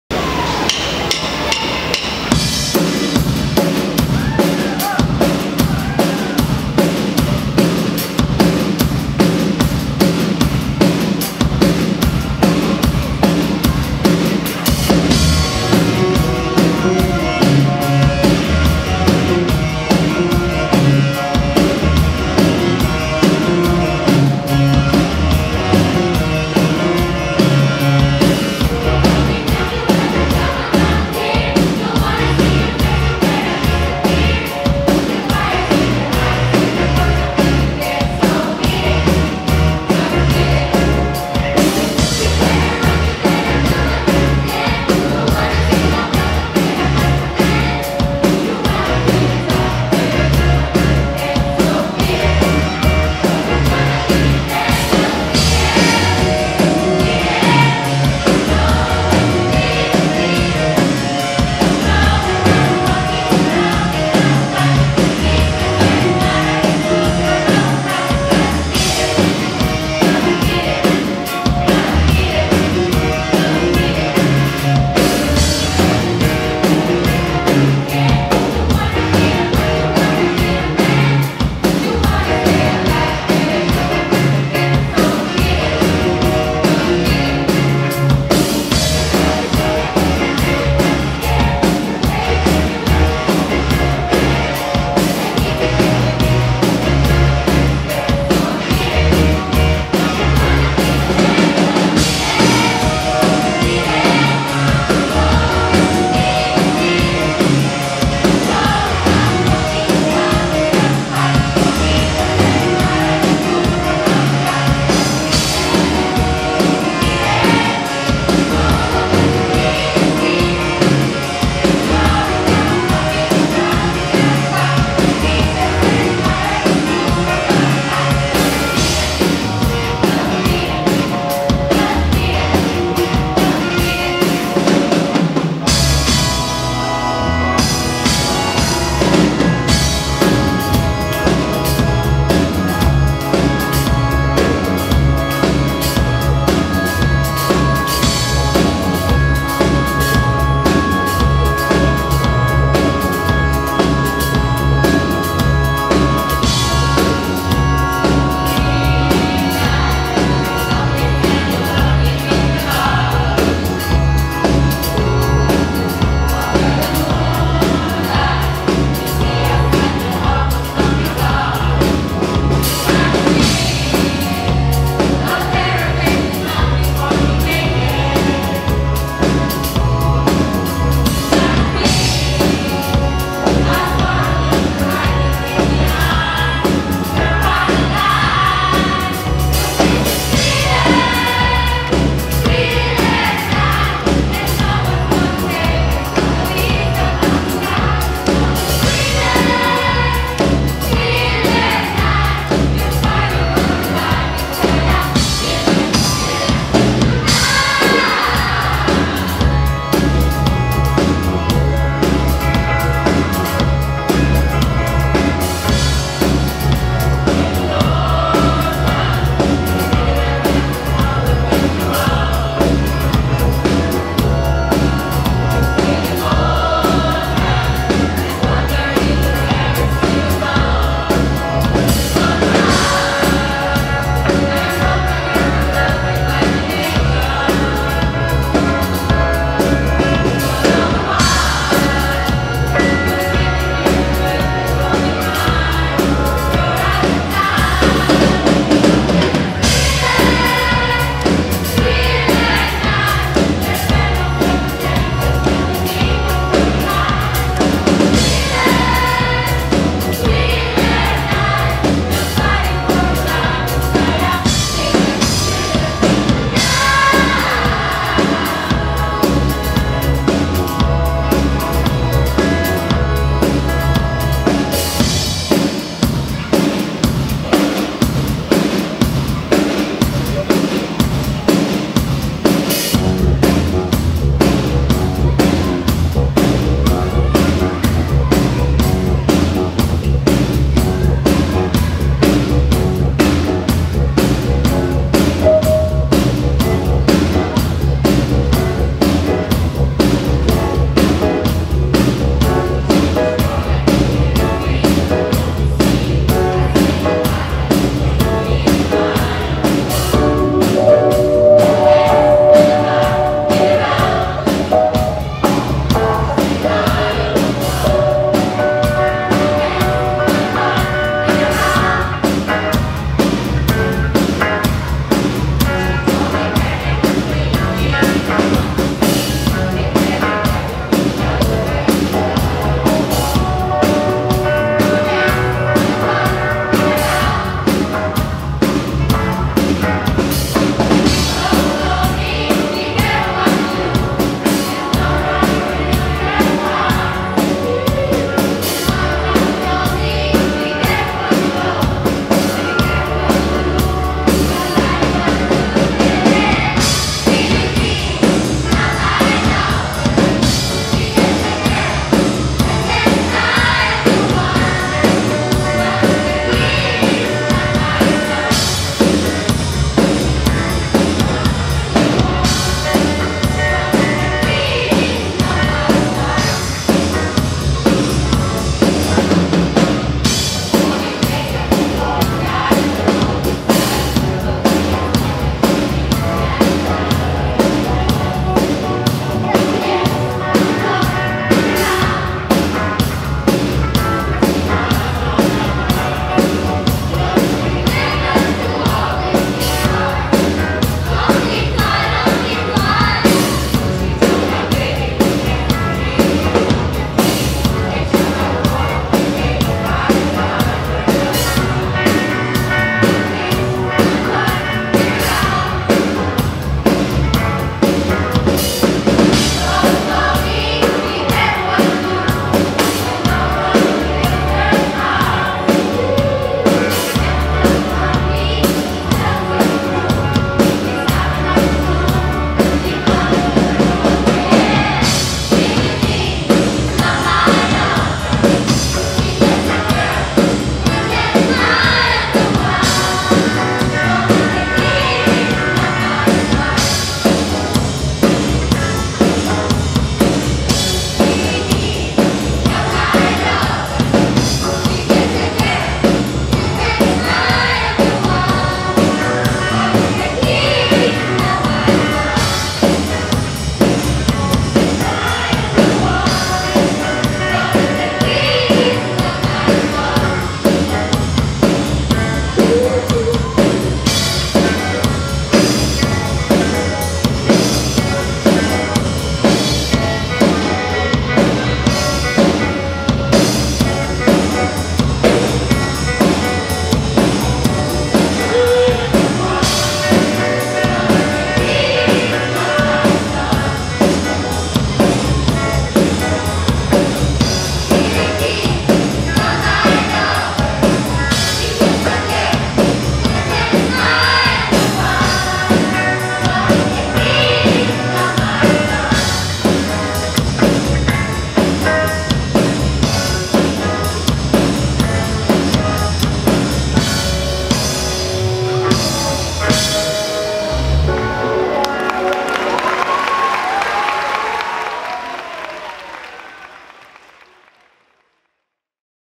Michael Jackson - Homenajes 2017
Oxford's Choir Performance 2017 @ Oxford School of English